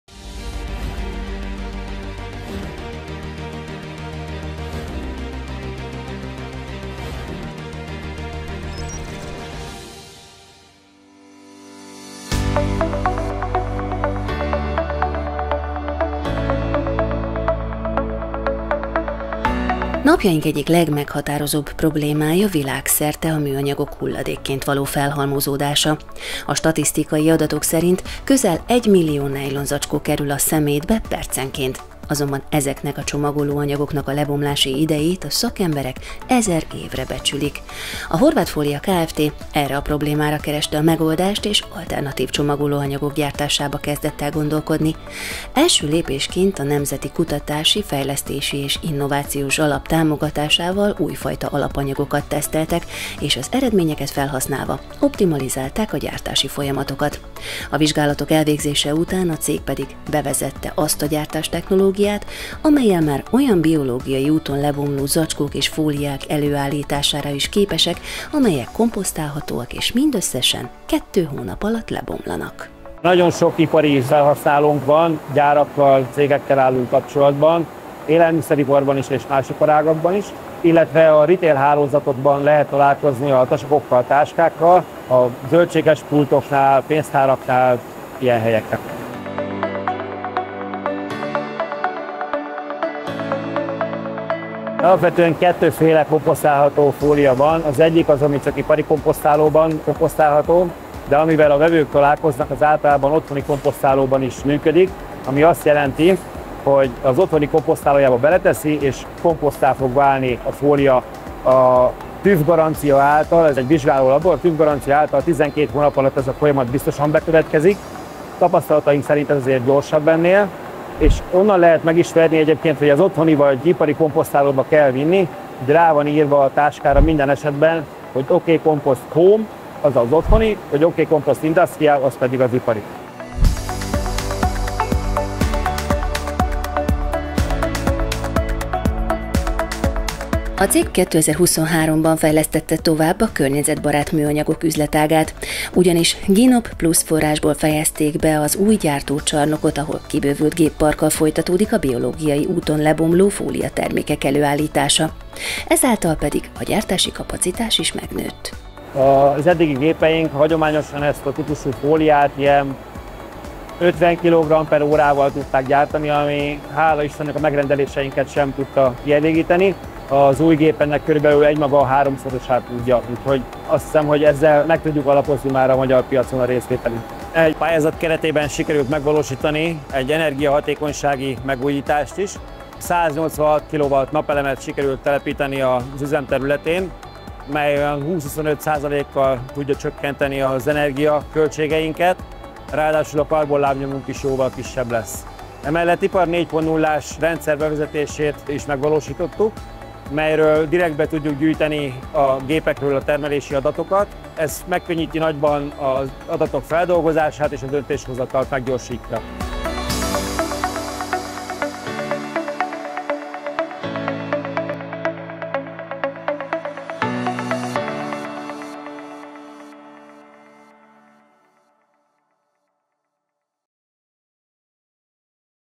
Műanyag csomagolás okosan
Unokáink sem fogják látni – jósolta borúsan a hajdanvolt városvédő tévéműsor. Mi magunk sem fogjuk látni – ez pedig már egy mai soproni kisvállalkozás szlogenje lehetne. Ám ez most kivételesen jó hír: az általuk gyártott, biológiailag lebomló fóliából készült csomagolóanyagok, műanyag zacskók ugyanis kevesebb, mint két hónap alatt „eltűnnek”, ha megfelelően komposztálják őket. A Nemzeti Kutatási, Fejlesztési és Innovációs Alapból támogatott fejlesztés sokat tesz a kereskedelemben használatos műanyag zacskók generációváltásáért, a csomagolóanyagok okozta környezetszennyezés csökkentéséért.